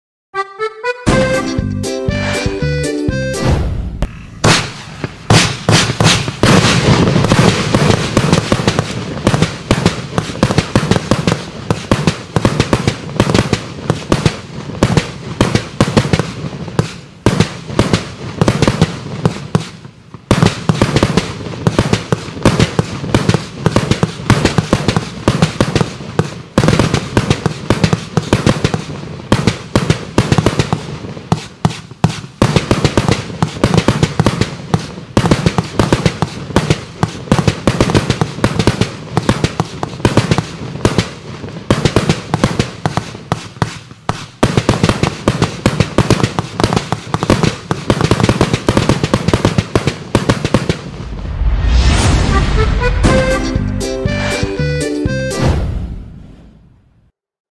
1.2'' Glitter Willow 2 Colorz vuurwerk | VOLT! | 100 shots professional compoundbox
De officiële video van de 1.2'' Glitter Willow 2 Colorz - 100 shots professional compoundbox

1.2'' Glitter Willow 2 Colorz: Connected - 100 shots profblok met mooie kleuren!

Specificaties 1.2'' Glitter Willow 2 Colorz:

Artikelnummer: 6502
Merk: VOLT! collectie
Vuurwerkimporteur: Lesli Vuurwerk
Categorie: Showboxen en Compounds 1 tot 2 Kg.
Aantal schoten: 100 shots
Kruitgewicht: 2000 gram
CE-Categorie: CAT F2
Afmeting: 360*360*180 mm
ADR-classificatie: 1.4G
Gewicht: 10,976 kg
Aantal per karton: 1
Tijdsduur: ca. 45 seconden
Maximale diameter tubes: 30 mm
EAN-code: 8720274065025

Straatfilm 1.2'' Glitter Willow 2 Colorz? Van het meeste vuurwerk vind je straatfilms in onze webshops. Check voor een vuurwerk webshop bij jou in de buurt!

Vind je dit filmpje van 1.2'' Glitter Willow 2 Colorz vuurwerk leuk en wil je meer zien?